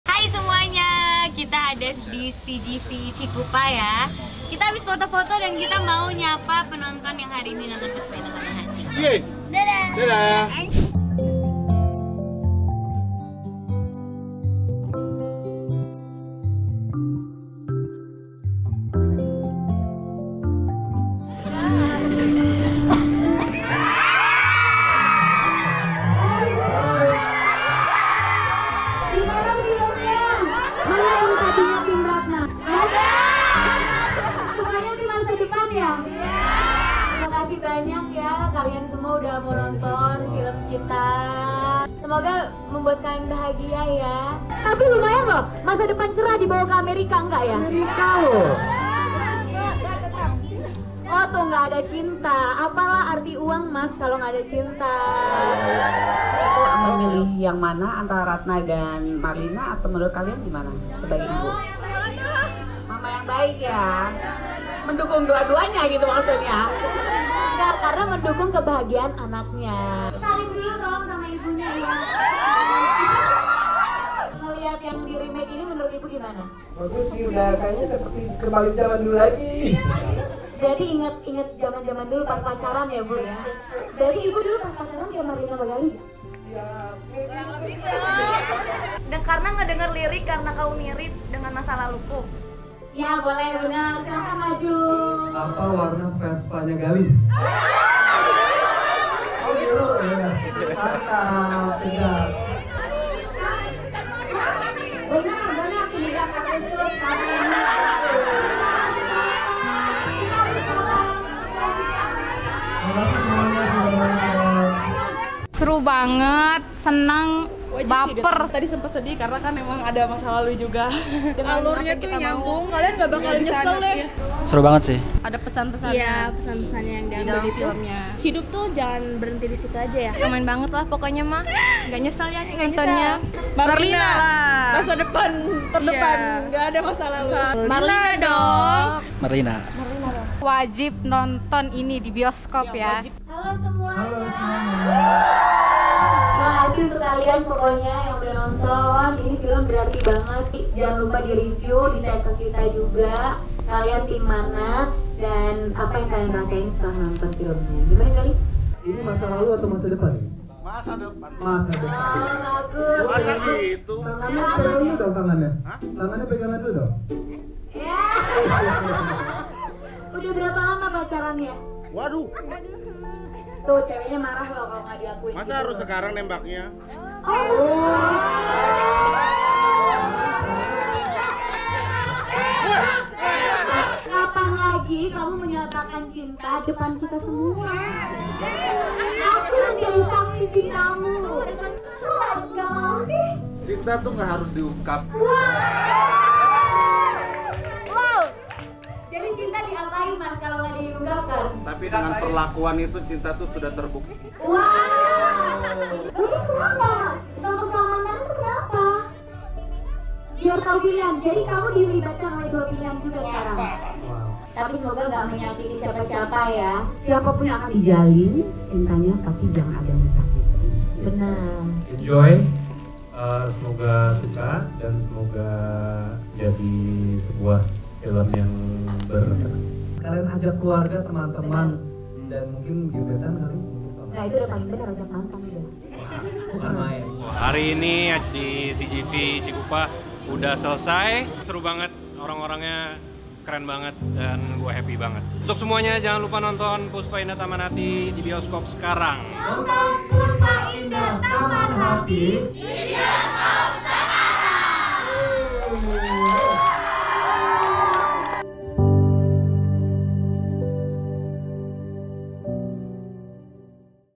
TANGERANG MAKIN GIRANG NONTON BARENG PUSPA INDAH TAMAN HATI
SAKSIKAN DI BIOSKOP

Diangkat dari novel legendaris karya Eddy D Iskandar

Produser : Chand Parwez Servia, Fiaz Servia
Sutradara : Monty Tiwa

Pemain:
Prilly Latuconsina, Yesaya Abraham, Dewi Gita, Lukman Sardi, Arnold Leonard, Vladimir Rama, Jasmine Nadya, Nala Amrytha, Sadha Triyudha, Ariyo Wahab, Elmayana Sabrenia, Shelomita, Dicky Wahyudi, Frans Nicholas, Barbie Arzetta, Emmie Lemu, Ozzol Ramdhan.